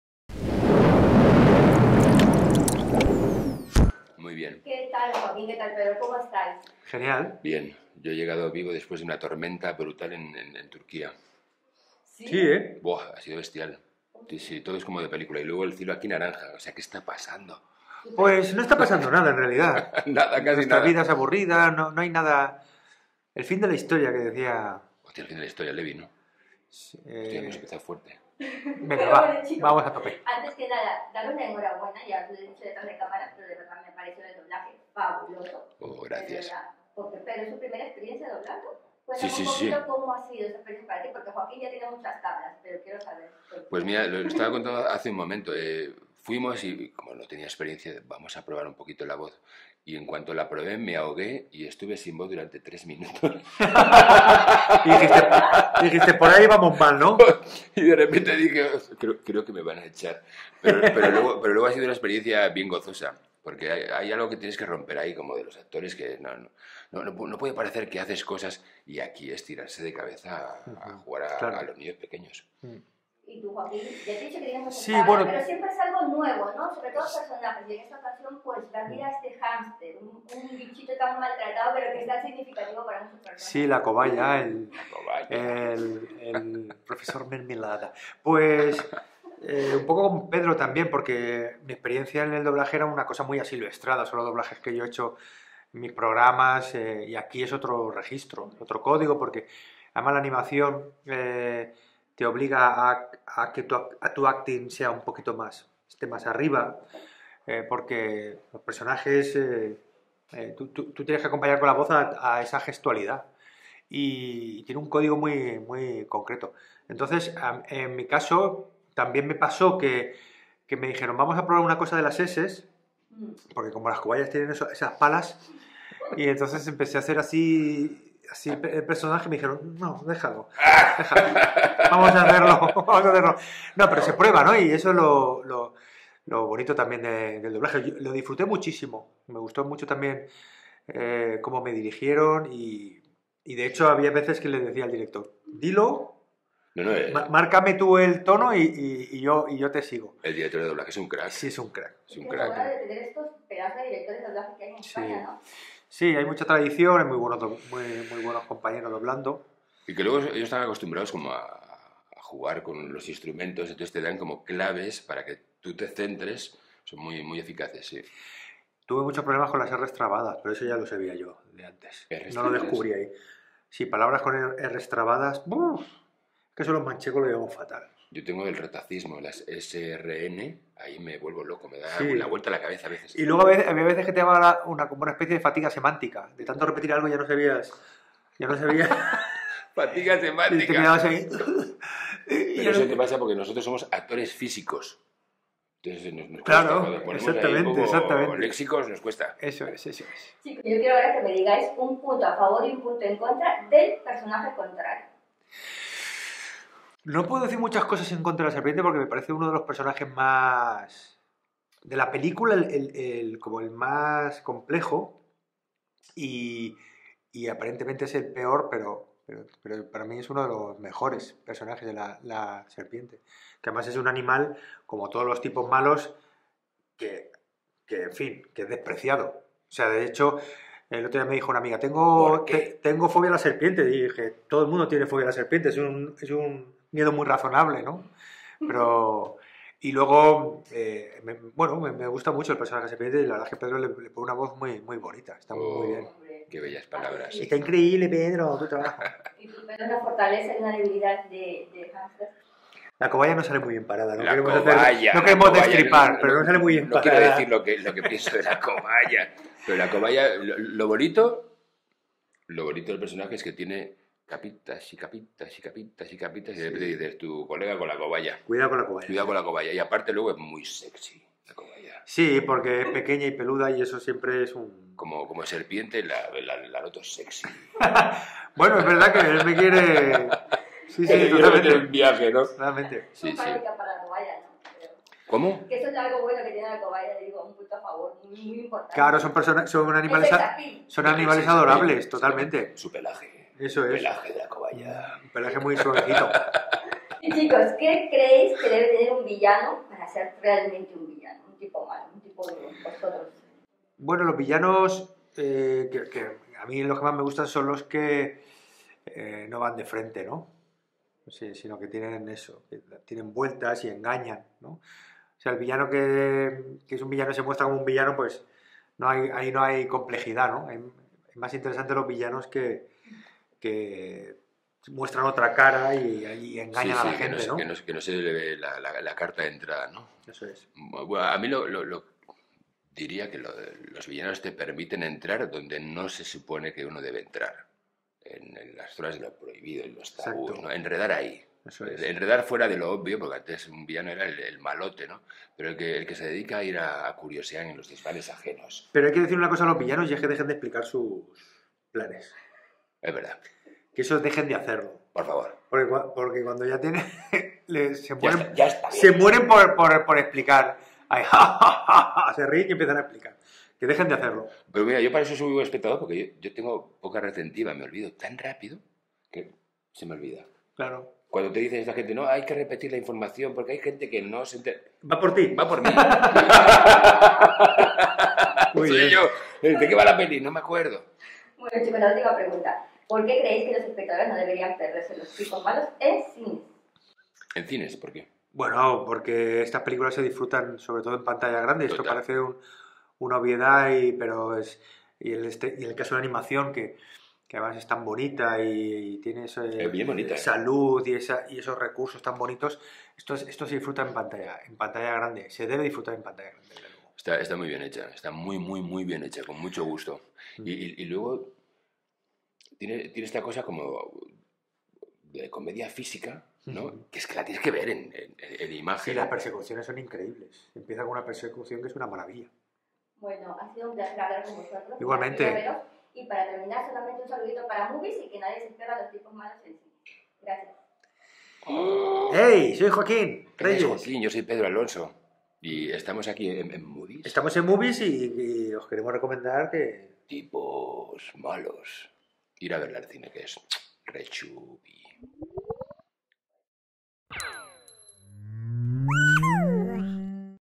Los tipos malos | Joaquín Reyes y Pedro Alonso: "los malos deberían de dejar de explicar sus planes"
Hablamos con Joaquín Reyes y Pedro Alonso que prestan sus voces en la nueva cinta de animación de @Universal_Spain , no te pierdas esta divertida charla.

Entrevista y edición: Verónica Dávila